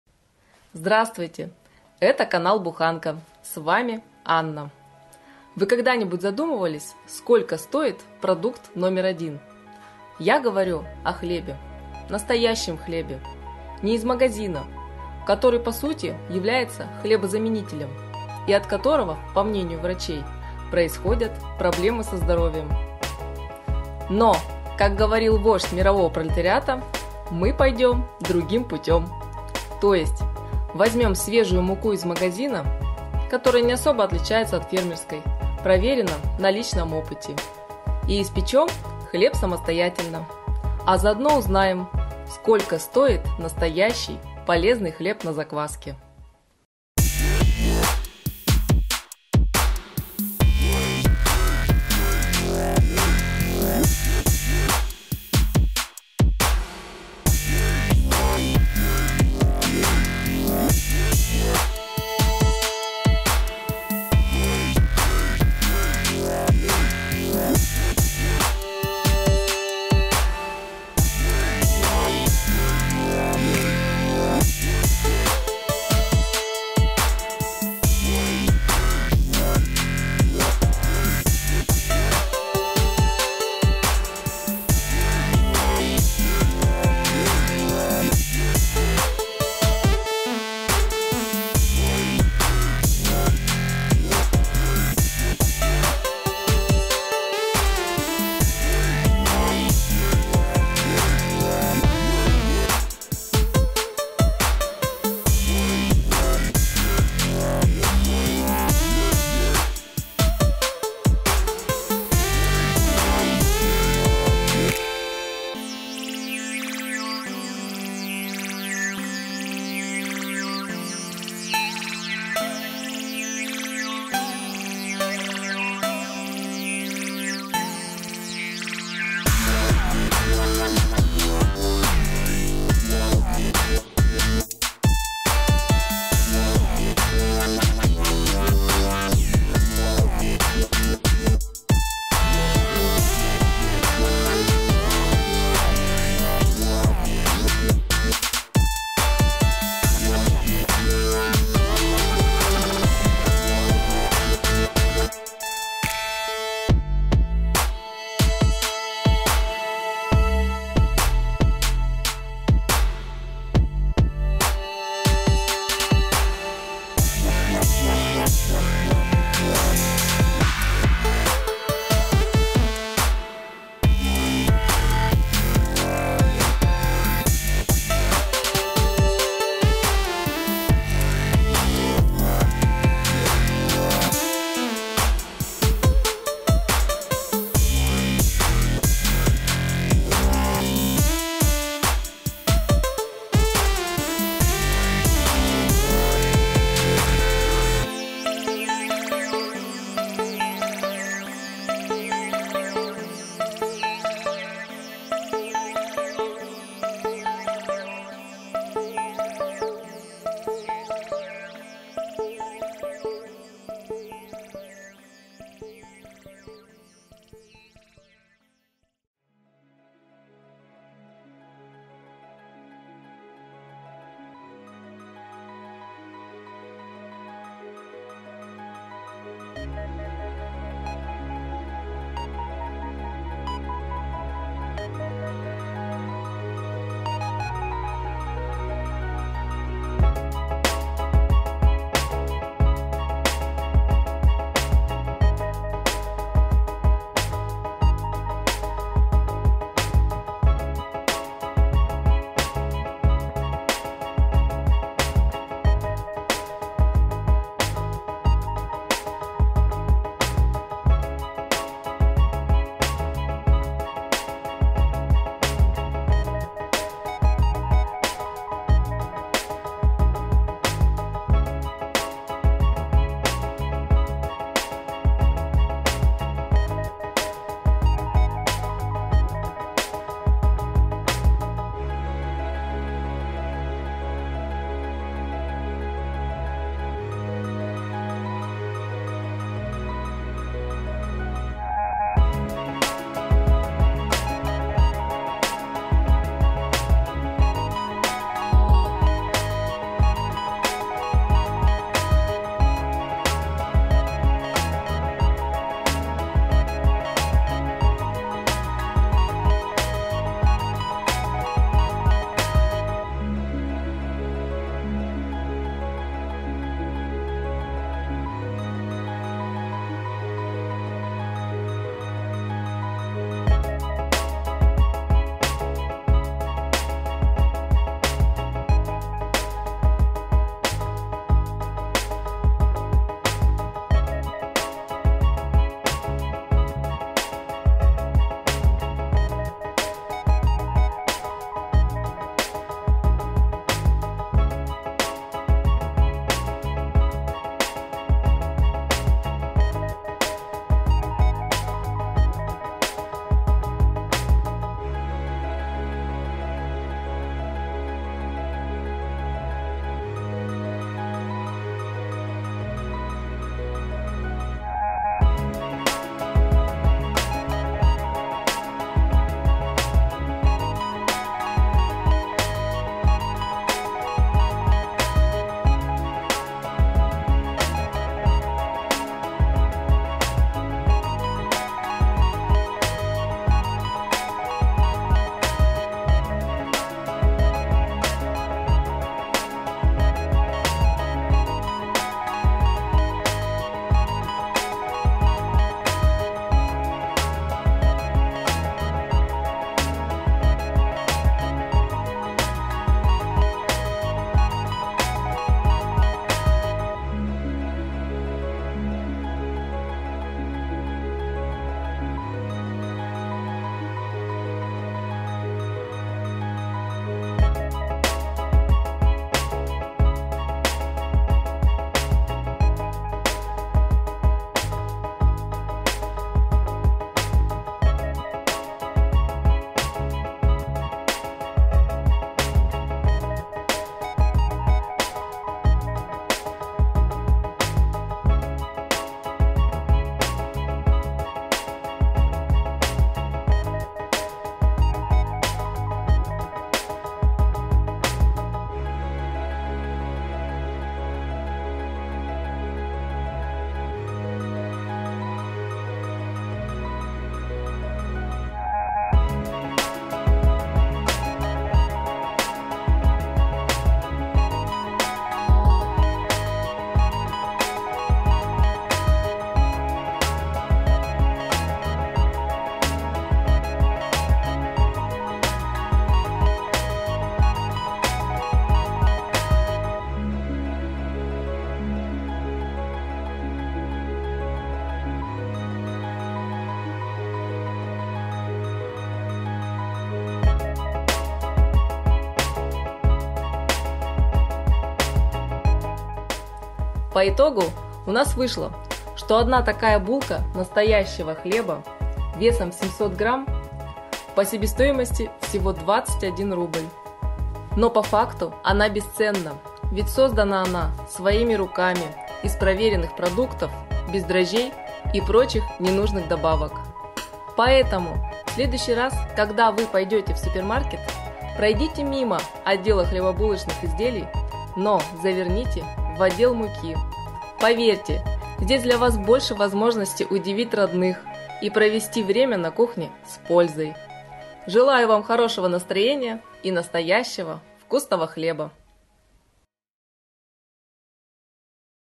Сколько стоит НАСТОЯЩИЙ ХЛЕБ?!
В этом видео я выявила цену настоящего хлеба на закваске. Во сколько обходится выпечка хлеба с учётом стоимости затраченных продуктов и потребляемой энергии!
Думаю это будет дополнительным плюсом в пользу выпечки домашнего хлеба на закваске!